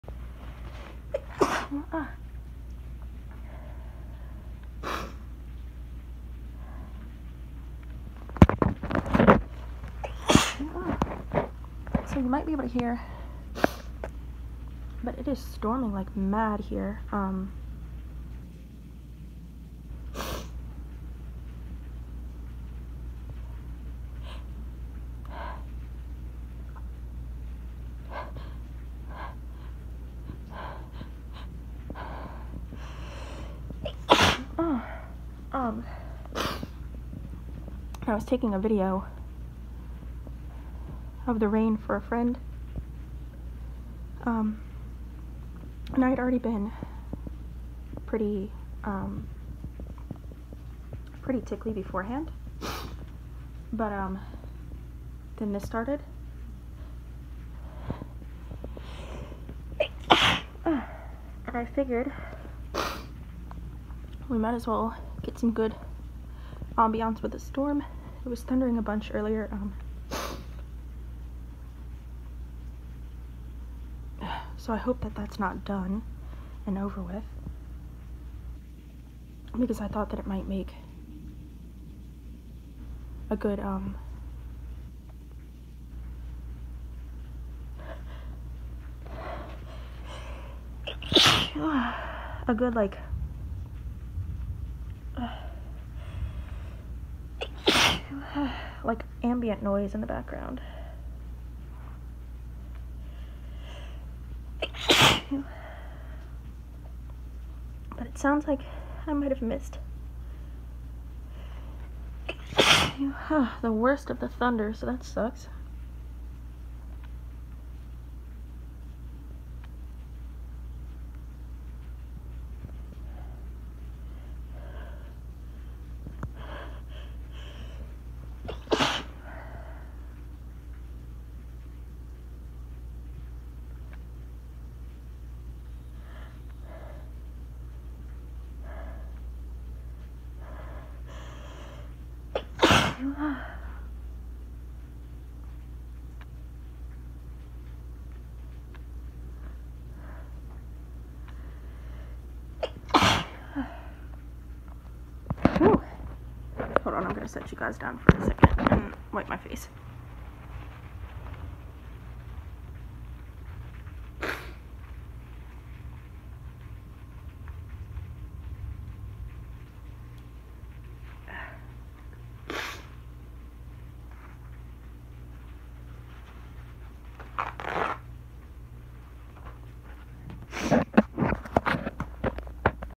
Rainy day small sneezing fit
Trying something new with this audio, but I’m gonna try getting a better mic because this is annoying :( I really just need a new phone but ¯\_(ツ)_/¯

It had stopped thundering :( but it should be back later on and with the way I’ve been going lately, I think my body might cooperate 😂 if need be I’ll just sniff something I’ll allergic to tbh.